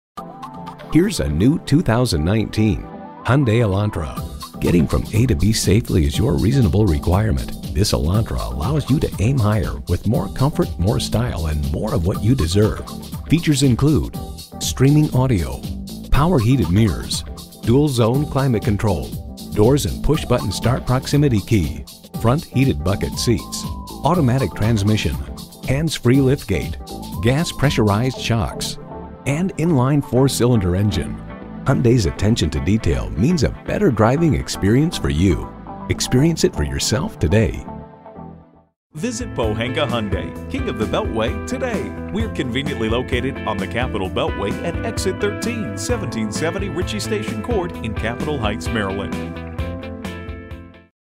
New 2019 Hyundai Elantra Capitol Heights MD Washington-DC, MD FKH427576 - SOLD
Year: 2019
Make: Hyundai
Model: Elantra
Trim: Value Edition
Engine: 2.0L 4-Cylinder
Transmission: Automatic
Color: Red
Interior: beige
Stock #: FKH427576
VIN: 5NPD84LF6KH427576
Scarlet Red 2019 Hyundai Elantra Value Edition FWD Automatic 2.0L 4-CylinderbrbrRecent Arrival! 28/37 City/Highway MPG Price includes: $500 - Military Program. Exp. 09/03/2019, $400 - College Graduate Program. Exp. Exp. 09/03/2019, $1,750 - Retail Bonus Cash. Exp. 09/03/2019, $500 - HMF Standard Rate Bonus Cash - CE, MA, SC, SO. Exp. 09/03/2019

Address:
1770 Ritchie Station Court